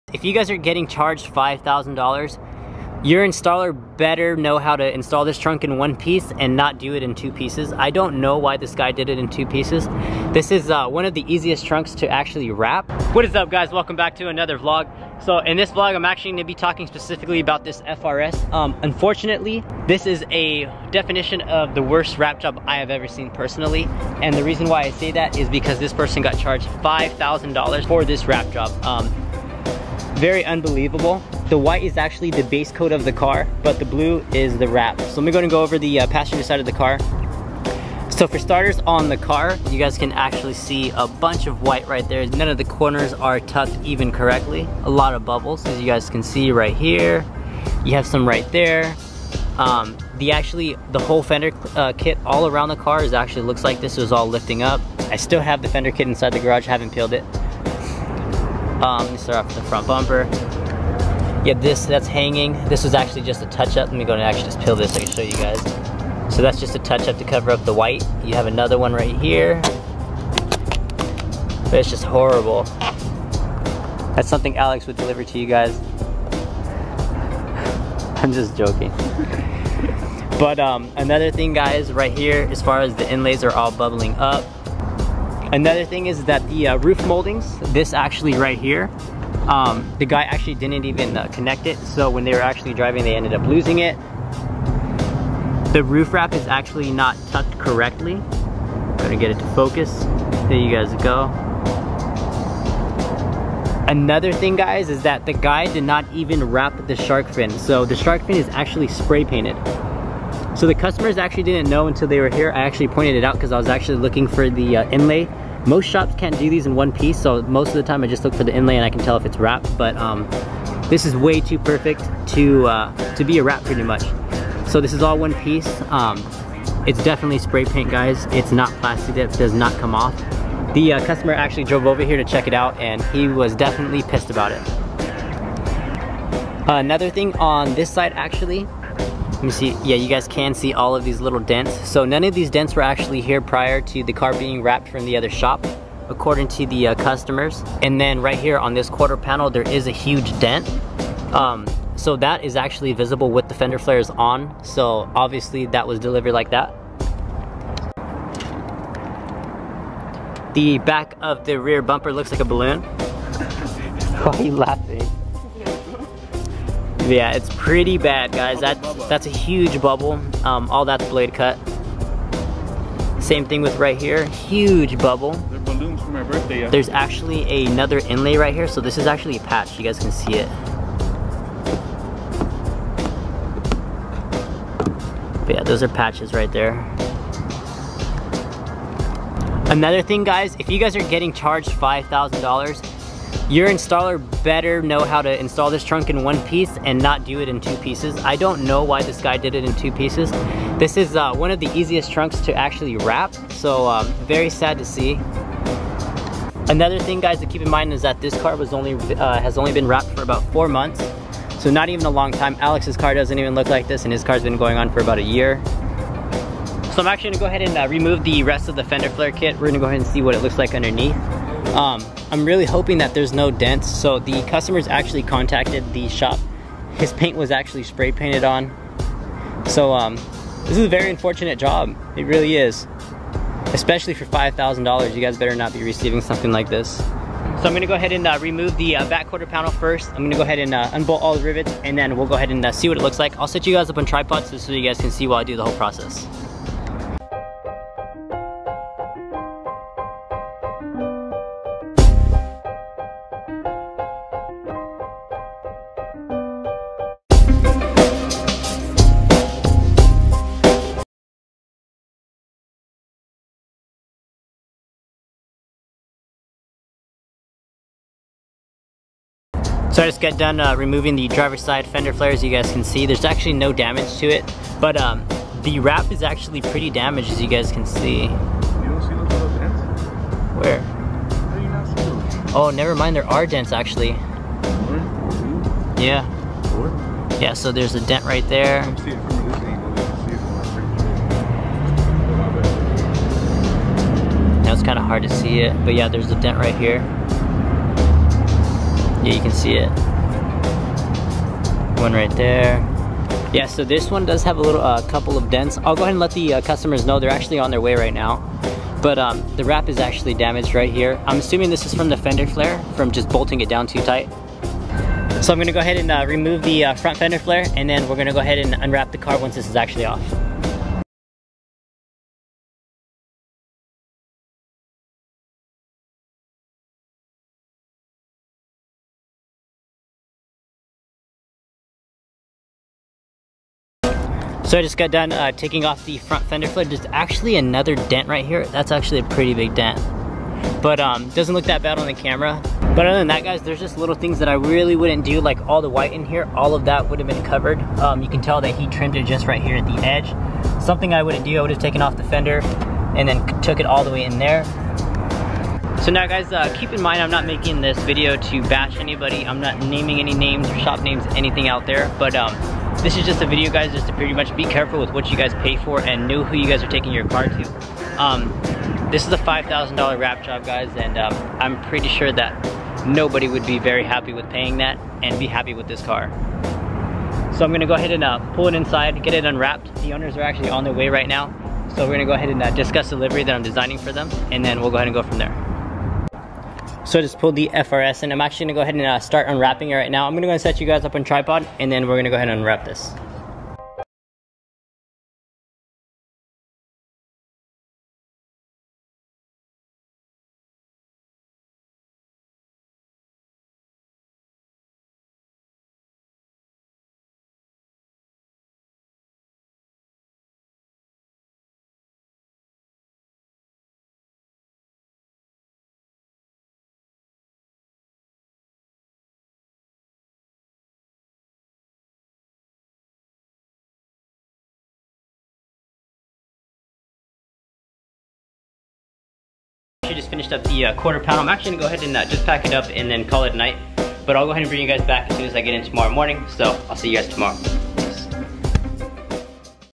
THE WORST WRAP JOB I HAVE EVER SEEN (Charged 5,000 Dollars For This)
Unfortunately this wrap community has been getting away with too much.